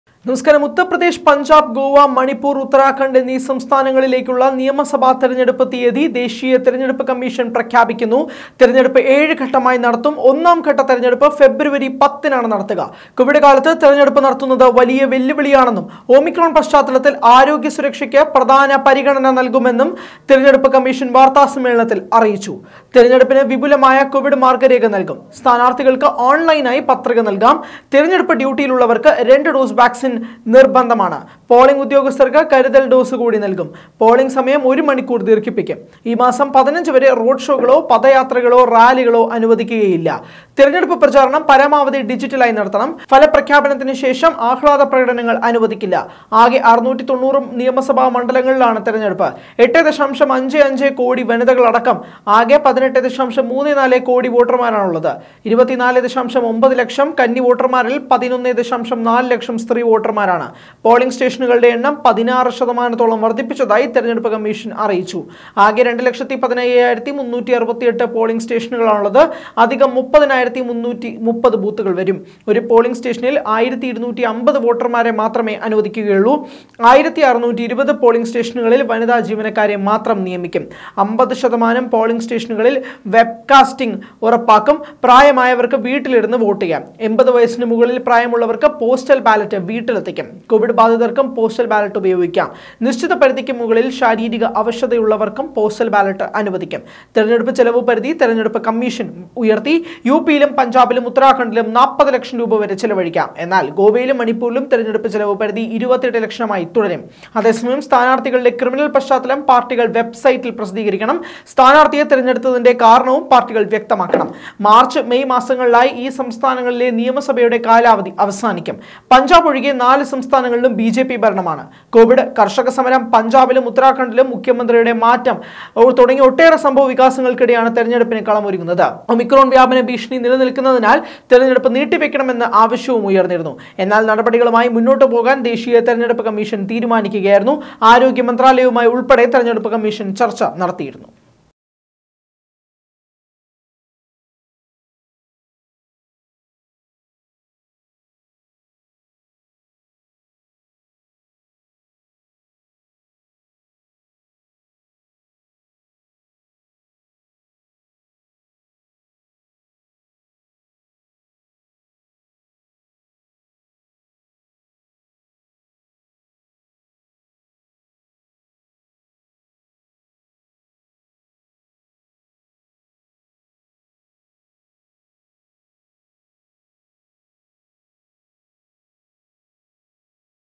7 ഘട്ടമായി തിരഞ്ഞെടുപ്പ്; ഫെബ്രുവരി 10ന് തുടക്കം I Election
ഉത്തർപ്രദേശ്, പഞ്ചാബ്, ഗോവ, മണിപ്പുർ, ഉത്തരാഖണ്ഡ് എന്നീ സംസ്ഥാനങ്ങളിലേക്കുള്ള നിയമസഭാ തിരഞ്ഞെടുപ്പ് തീയതി ദേശീയ തിരഞ്ഞെടുപ്പ് കമ്മിഷൻ പ്രഖ്യാപിക്കുന്നു. തിരഞ്ഞെടുപ്പ് 7 ഘട്ടമായി നടത്തും. ഒന്നാം ഘട്ട തിരഞ്ഞെടുപ്പ് ഫെബ്രുവരി പത്തിന്. കോവിഡ് കാലത്ത് തിരഞ്ഞെടുപ്പ് നടത്തുന്നത് വലിയ വെല്ലുവിളിയാണെന്നും ഒമിക്രോൺ പശ്ചാത്തലത്തിൽ ആരോഗ്യ സുരക്ഷയ്ക്ക് പ്രധാന പരിഗണന നൽകുമെന്ന് തിരഞ്ഞെടുപ്പു കമ്മിഷൻ വാർത്താ സമ്മേളനത്തിൽ അറിയിച്ചു.